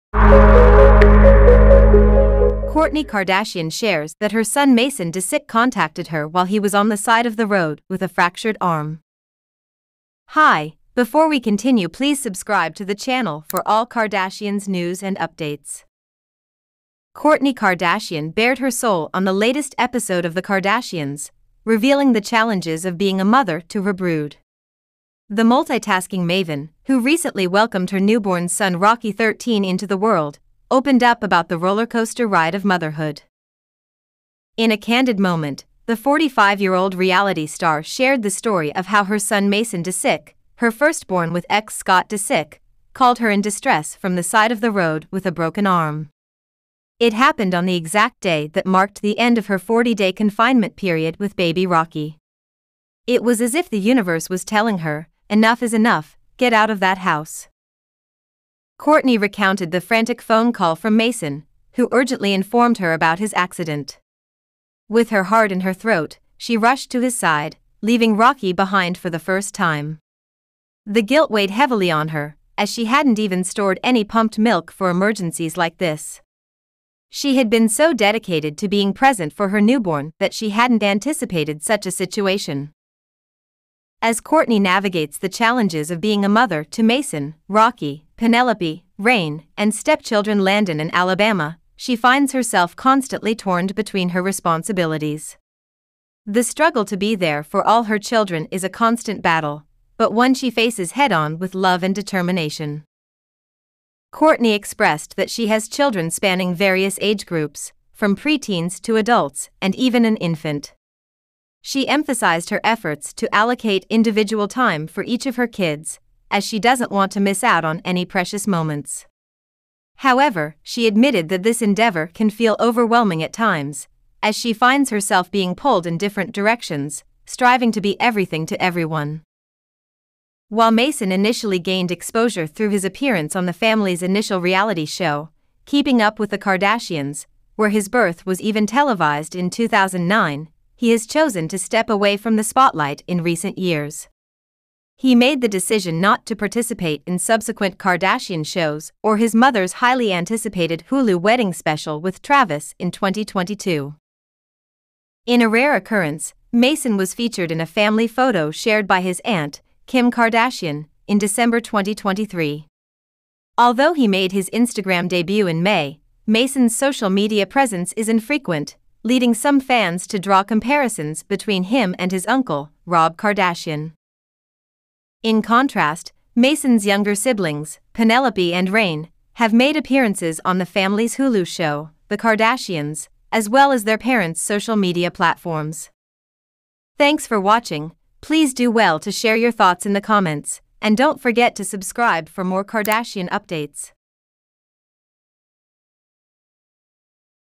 DNA EXPOSED! Kourtney K Explains TRUTH After DNA PROVES Scott Disick Not Mason Disick Father
Kourtney Kardashian shares that her son Mason Disick contacted her while he was on the side of the road with a fractured arm.

Kourtney Kardashian bared her soul on the latest episode of 'The Kardashians', revealing the challenges of being a mother to her brood. The multitasking maven, who recently welcomed her newborn son Rocky Thirteen into the world, opened up about the rollercoaster ride of motherhood.

In a candid moment, the 45-year-old reality star shared the story of how her son Mason Disick, her firstborn with ex Scott Disick, called her in distress from the side of the road with a broken arm. It happened on the exact day that marked the end of her 40-day confinement period with baby Rocky. It was as if the universe was telling her, "Enough is enough, get out of that house!"

Kourtney recounted the frantic phone call from Mason, who urgently informed her about his accident. With her heart in her throat, she rushed to his side, leaving Rocky behind for the first time. The guilt weighed heavily on her, as she hadn't even stored any pumped milk for emergencies like this. She had been so dedicated to being present for her newborn that she hadn't anticipated such a situation.

As Kourtney navigates the challenges of being a mother to Mason, Rocky, Penelope, Reign, and stepchildren Landon and Alabama, she finds herself constantly torn between her responsibilities. The struggle to be there for all her children is a constant battle, but one she faces head-on with love and determination.